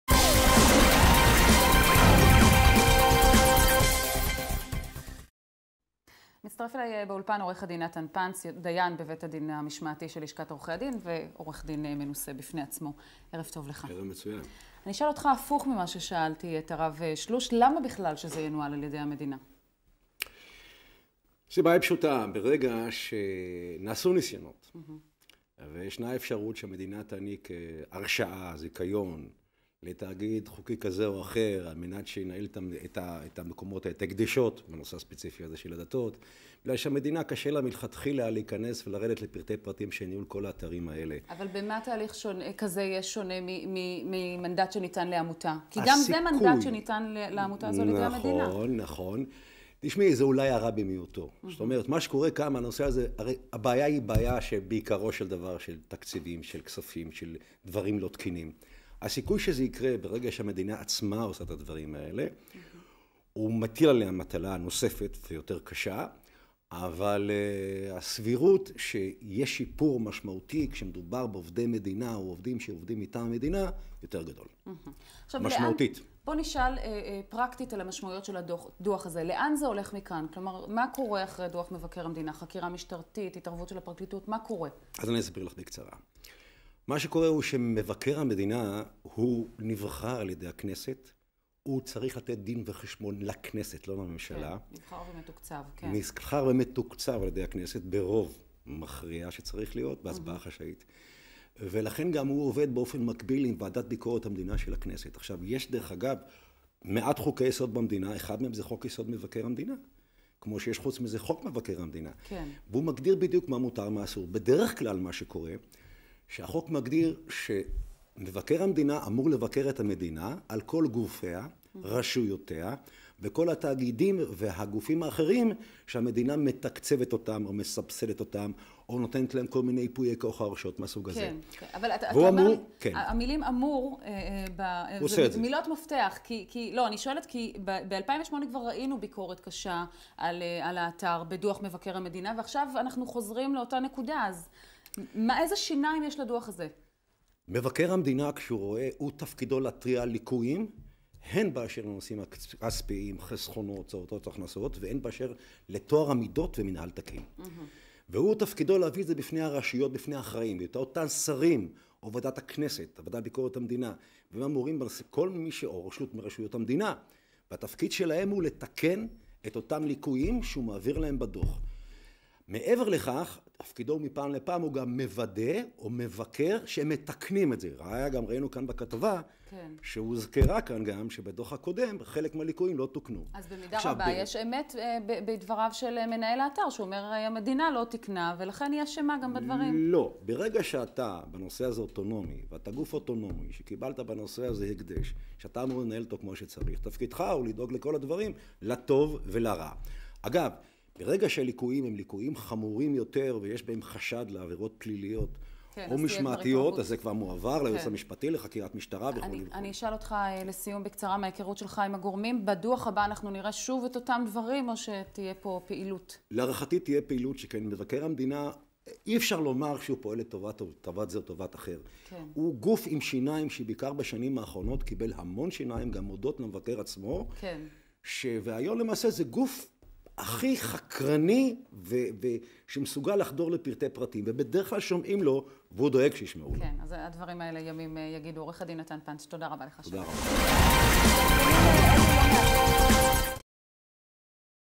ראיון עם עו"ד נתן פנץ
בעקבות דו"ח מבקר המדינה: עו"ד נתן פנץ על ההיבטים המשפטיים של הדו"ח, מי אחראי לתיקונים וכמה סמכות יש בכלל בידי המבקר